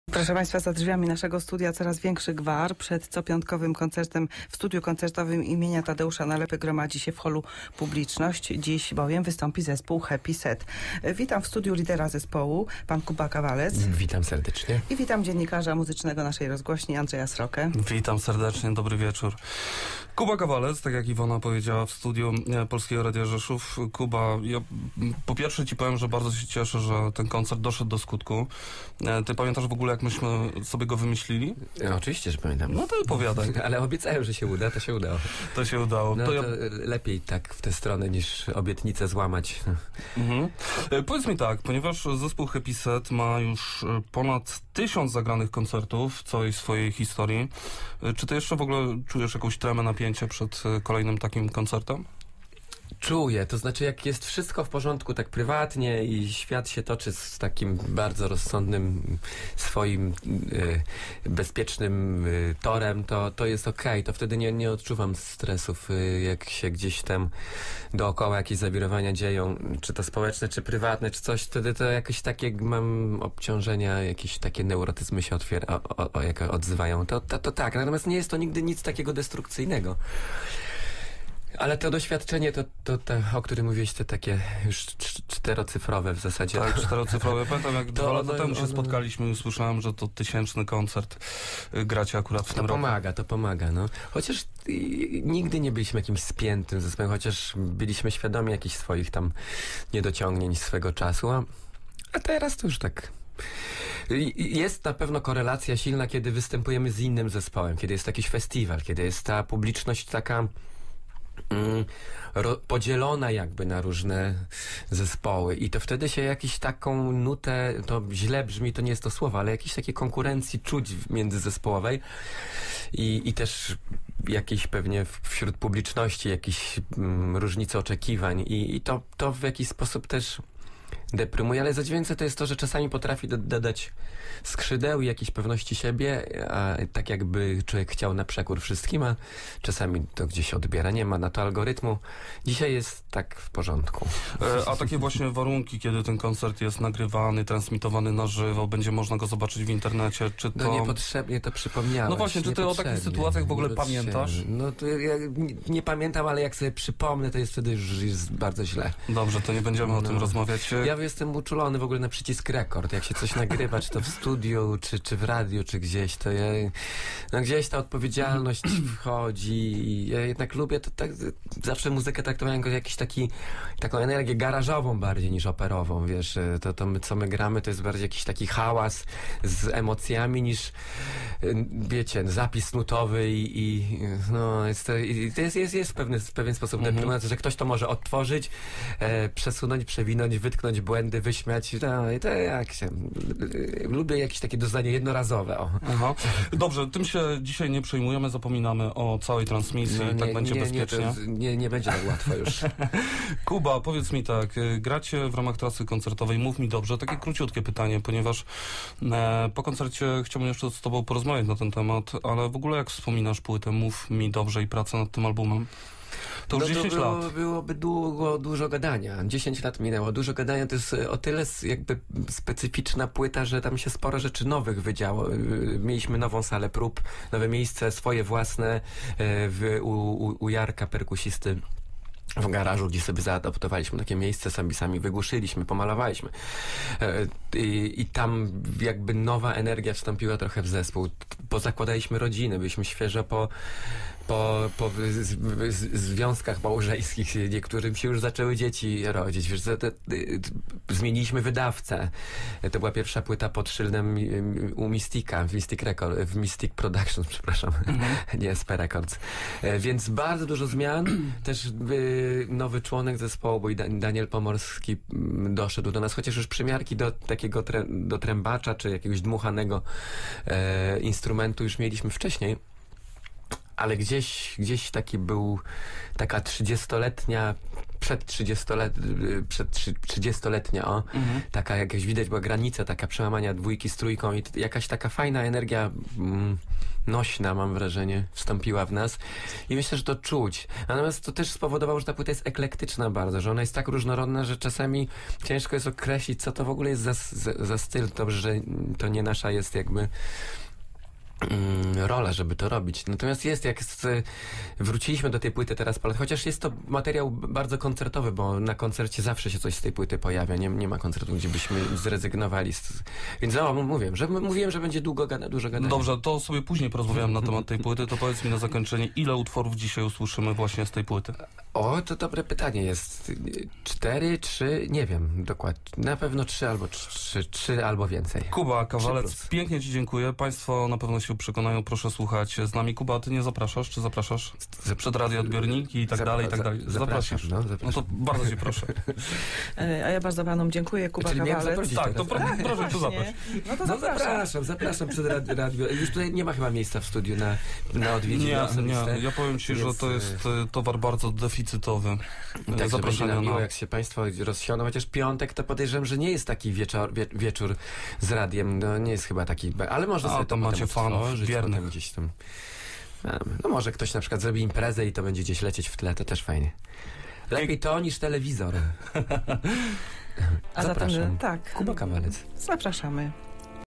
happysad przed koncertem - rozmawia red. Andrzej Sroka
Tuż przed koncertem z zespołem happysad rozmawiał Andrzej Sroka.